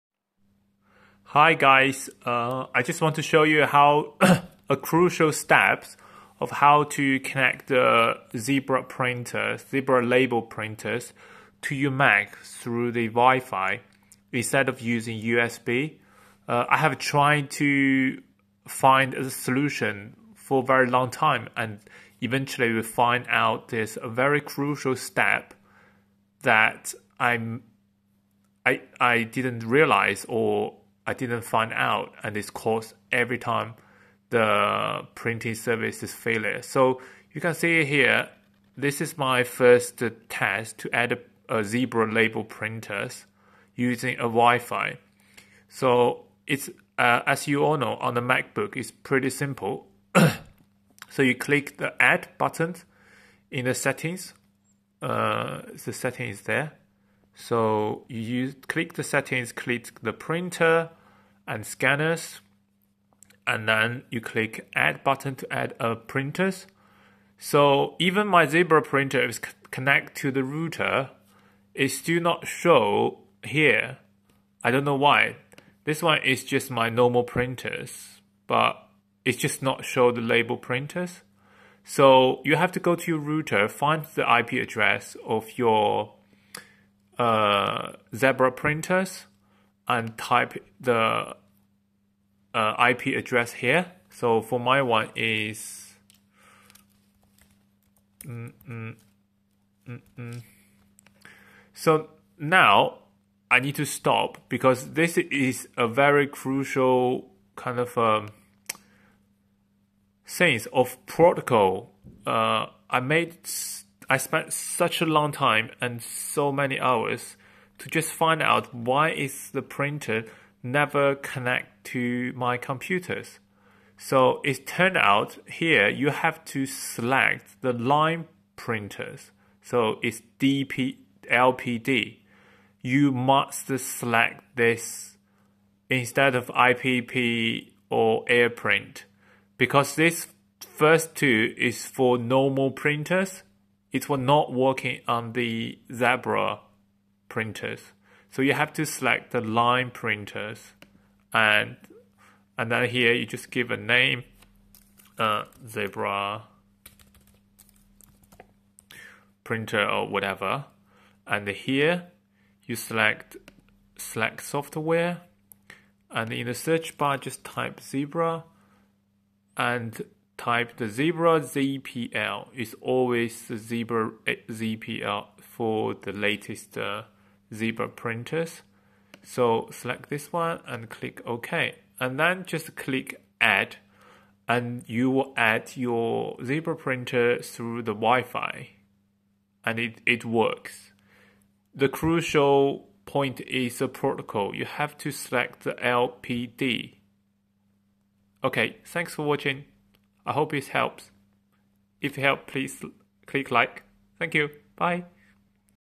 How to connect Zebra gk420d label printer with macbook via wifi - the key step
Trouble shooting: The printer is not responding.

Can't find the printer. Can't connect the printer